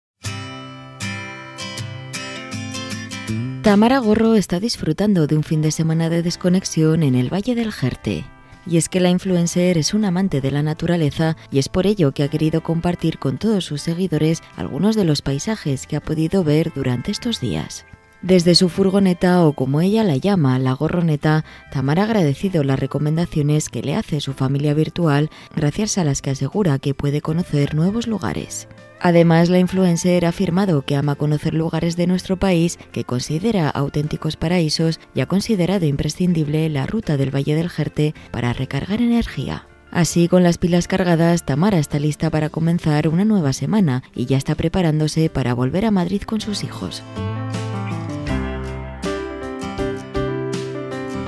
Tamara Gorro disfruta de un 'finde' de desconexión en el Valle del Jerte
Tamara Gorro está disfrutando de un fin de semana de desconexión en el Valle del Jerte. Y es que la influencer es una amante de la naturaleza, y es por ello que ha querido compartir con todos sus seguidores algunos de los paisajes que ha podido ver durante estos días.

(Fuente: Instagram de Tamara Gorro)